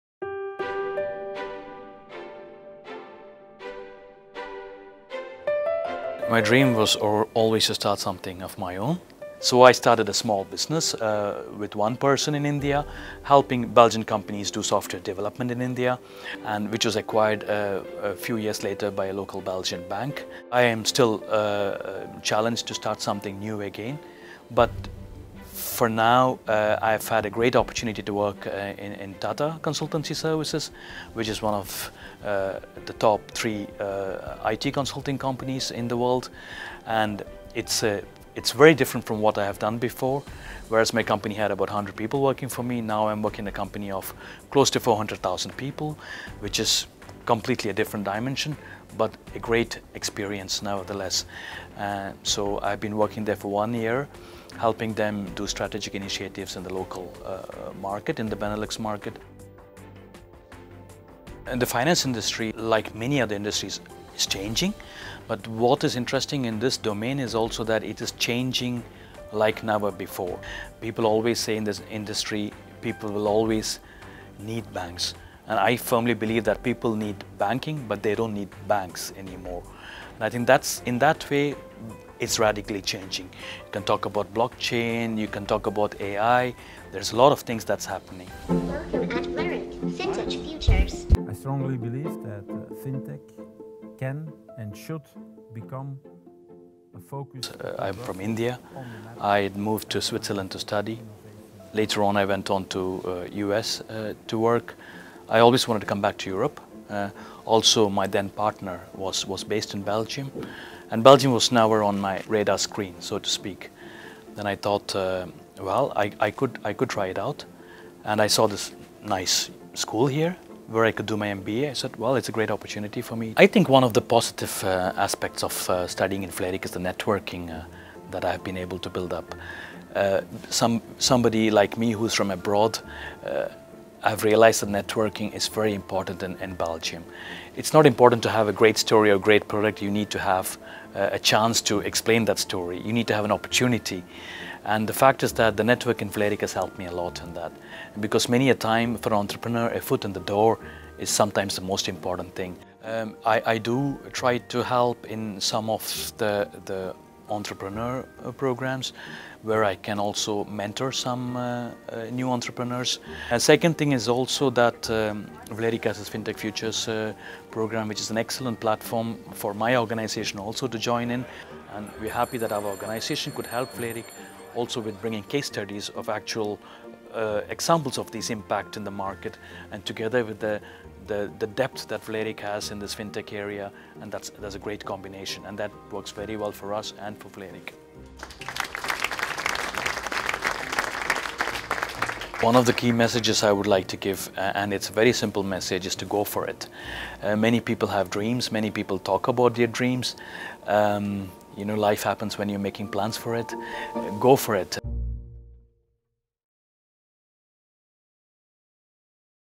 Entrepreneurs need to get their foot in the door
Bipin Nambiar always dreamed of starting a business of his own. So, he launched a small business in India, helping Belgian companies develop software. When his company was acquired by a Belgian bank, he looked for new ways to satisfy his entrepreneurial spirit.

Today, he’s working for Tata Consultancy Services (TCS) − a multinational IT service, consulting and business solutions company, headquartered in Mumbai, India. One of the 3 largest IT consultancies in the world, TCS is on a much larger scale than what Bipin experienced before: his own company employed about 100 people; now he’s in an enterprise of close to 400,000 employees worldwide. “A completely different dimension, but a great experience!” he says happily.

Bipin Nambiar: “One of the very positive aspects of studying at Vlerick is the network that you develop. Networking is very important, especially for someone like me who is from abroad. You not only need a great product, you need to have an opportunity to explain that product. Getting a foot in the door is sometimes the most important thing for an entrepreneur − and the Vlerick network has really helped me in that.”